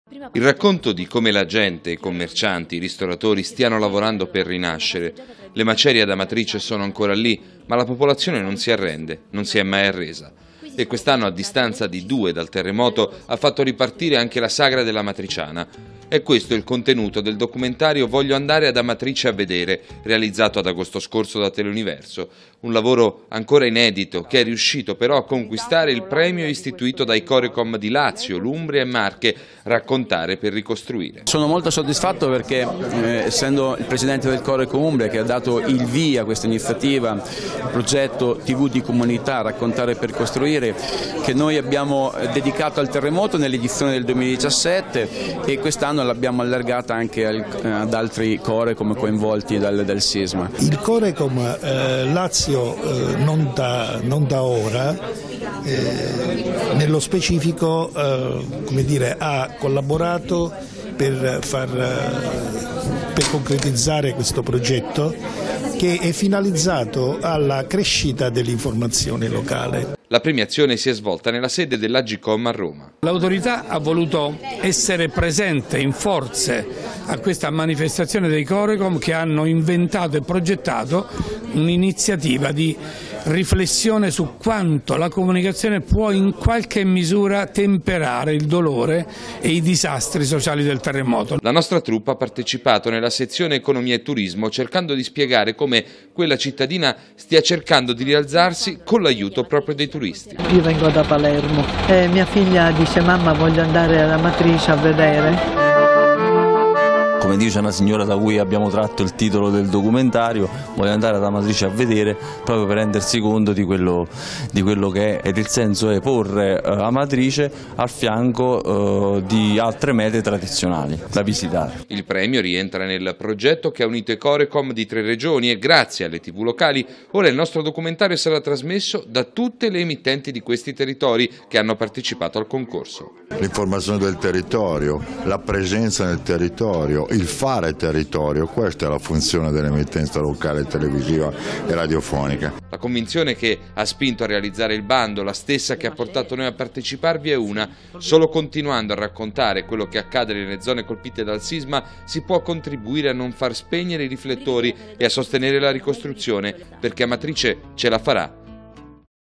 ROMA: Premio Corecom a Teleuniverso del 19-12-2018
Ci era stato anticipato nella festa per i nostri 40 anni, ma oggi c'è stata la consegna ufficiale. Teleuniverso, grazie al lavoro di Lorenza Di Brango e Franz Petrò Falese, ha vinto il premio indetto dai Corecom di Lazio, Umbria e Marche dedicato al sisma del 2016.